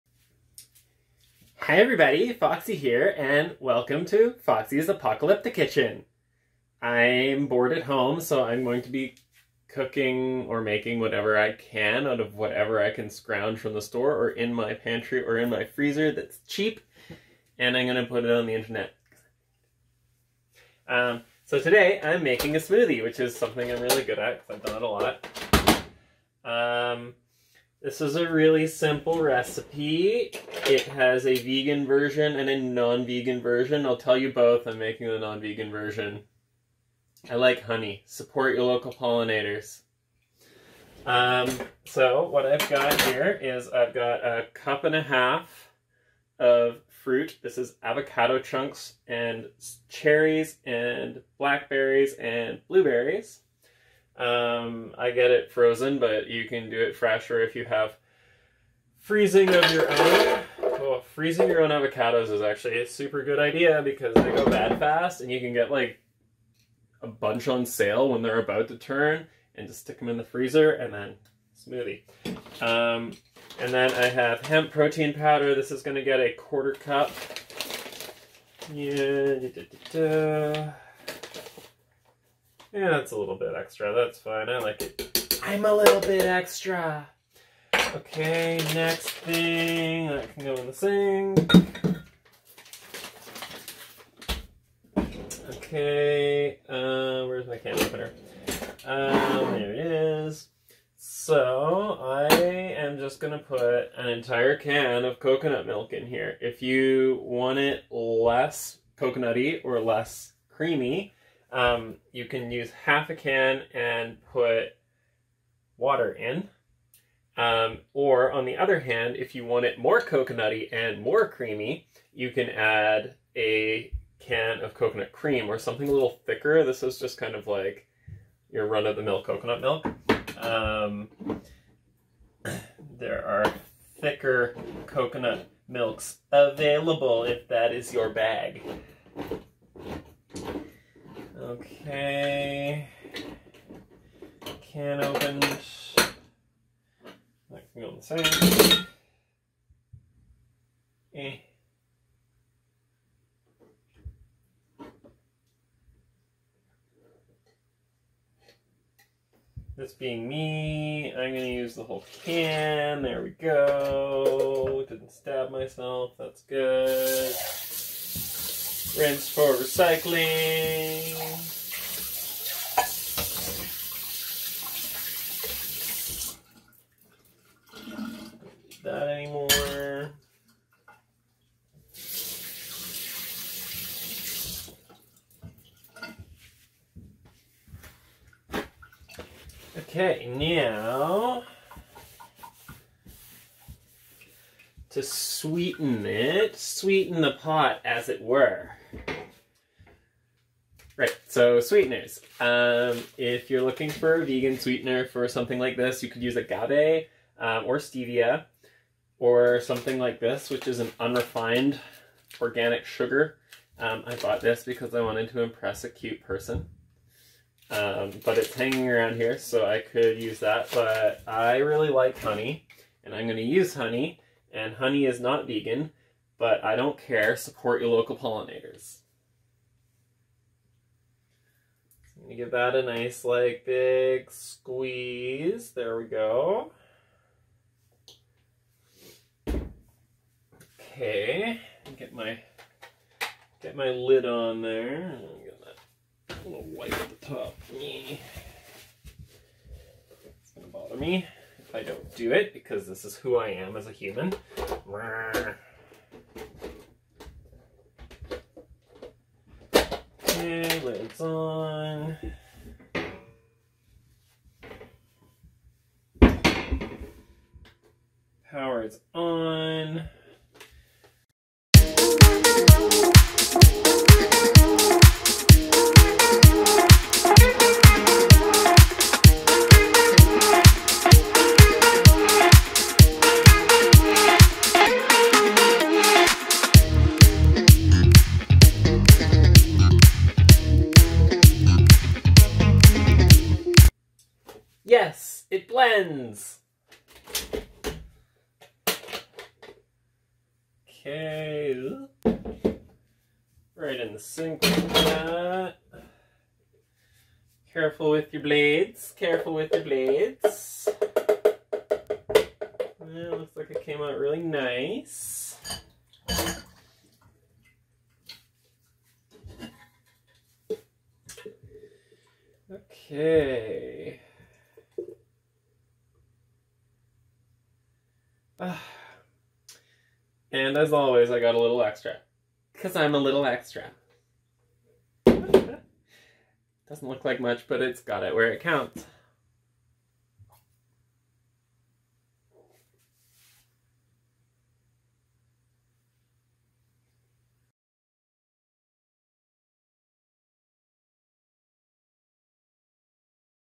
FoxxC's ApocalyptiKitchen #1 - CocoBerry Avocapocalypse
Hi everyone!  FoxxC here with a new bit of fun while we're all indoors: ApocalyptiKitchen!

I'm going to make some simple but tasty recipes with whatever I have on hand or can scrounge up at the store and share them with you here!

This first one is a simple smoothie, but its delicious and packs a lot of fats and proteins into a small cup. Enjoy!